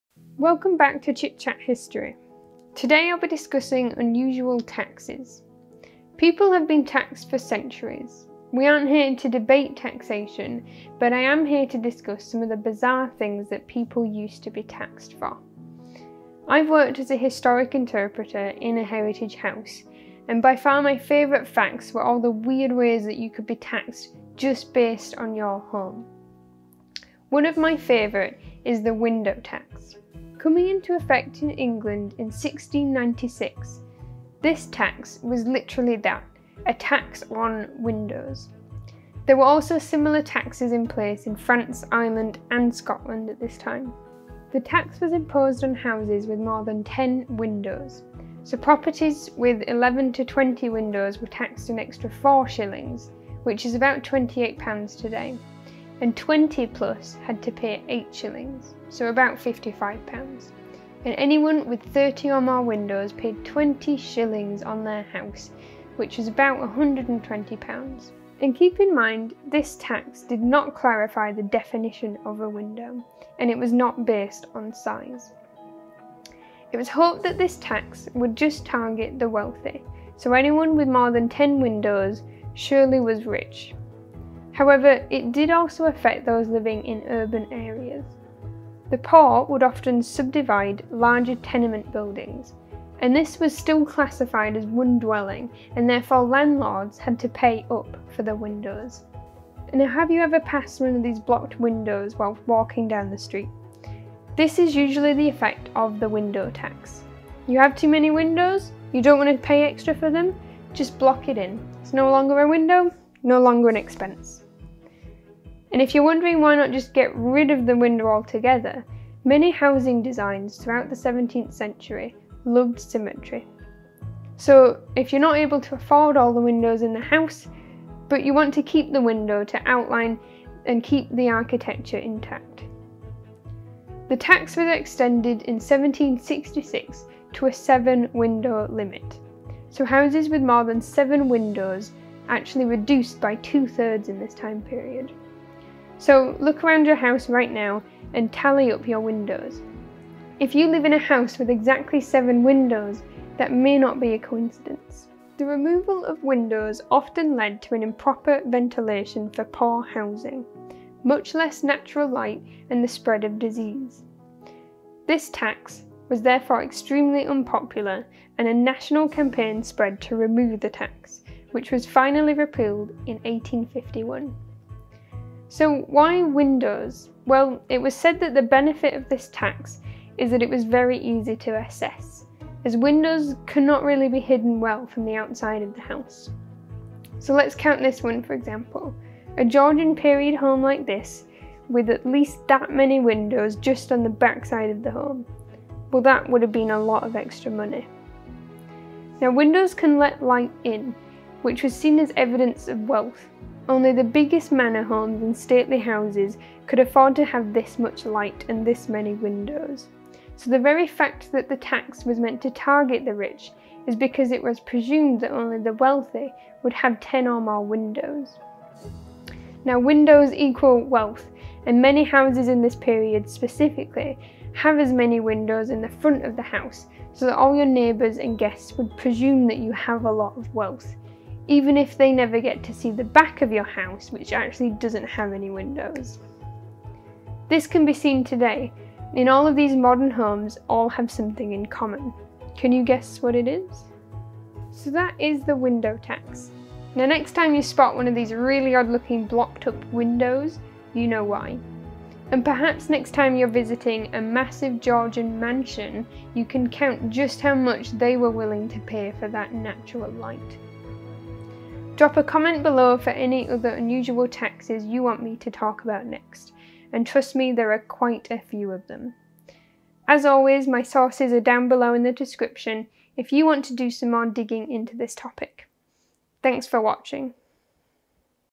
Blocked windows of England - Unusual Taxes
Have you ever wondered why some of the windows are blocked in a particular old building in England? Well it is most likely because of the Window Tax.
Coming into effect in England in 1696 -this Tax was literally that -a tax on windows. Very unusual tax indeed.
Discussing unusual taxes of England, first up on this list is the Window Tax. This tax is unique enough that it left a lasting remark on the design of houses for generations. If that wasn't enough, you can still see the effects of this tax to this very day. This tax was adopted in some form in Scotland, Ireland and France, you may be able to spot many blocked windows in these places and now know why!
Do you know any other unusual taxes other than the Window Tax you want us to discuss?
Chit Chat History.

Andrew E. Glantz (2008). "A tax on light and air: Impact of the Window Duty on Tax Administration and Architecture, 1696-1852". University of Pennsylvania. Retrieved 8 September 2015.